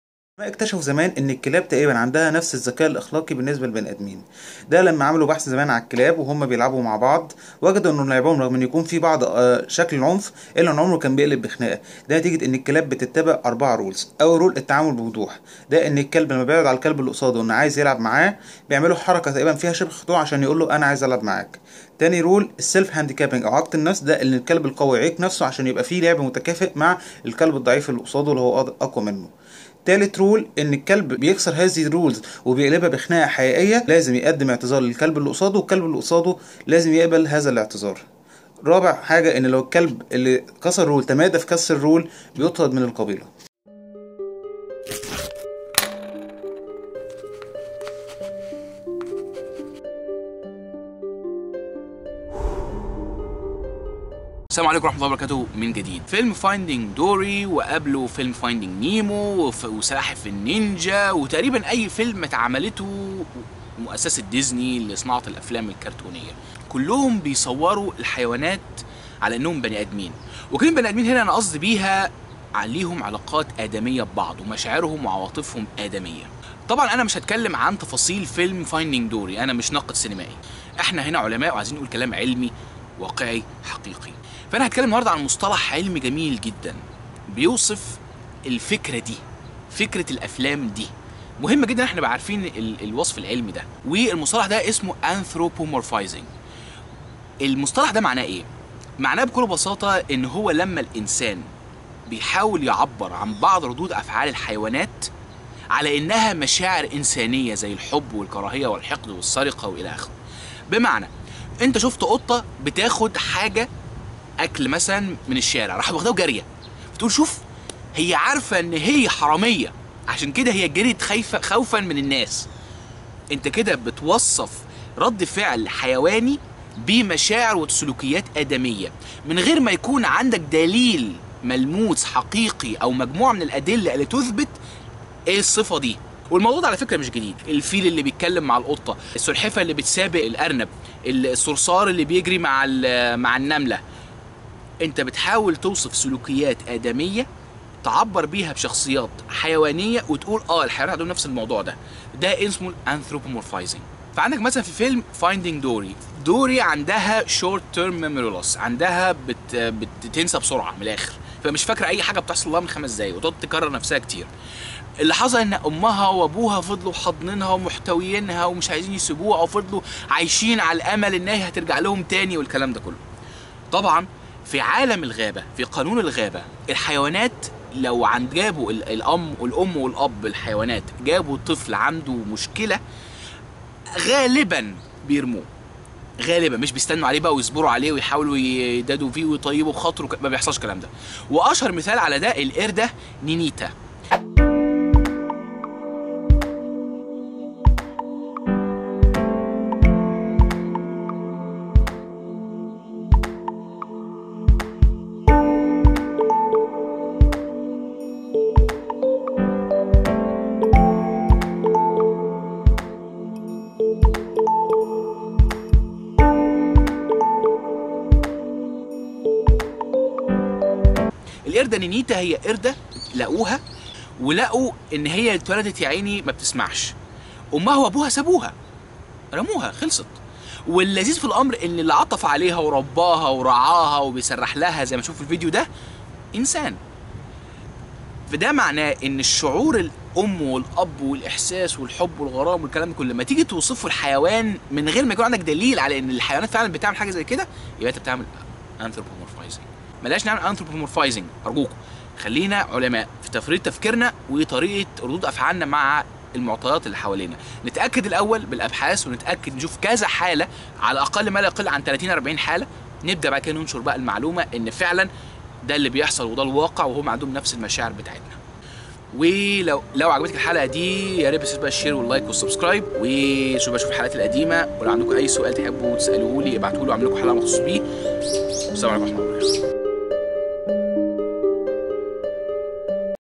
العلم وراء فيلم البحث عن دوري - anthropomorphism
الanthropomorphism عادة بشرية من قديم الأزل .. لكن ما معني هذا المصطلح؟ و ما علاقة فيلم "البحث عن دوري" بهذا المصطلح؟ كل هذا و أكثر في هذه الحلقة من معلومة علي الهامش :)

إعداد و تقديم: محمود عمرو النجار

تويتر: Twitter: @knowledge_caps
إنستاجرام: Instagram: @knowledgecaps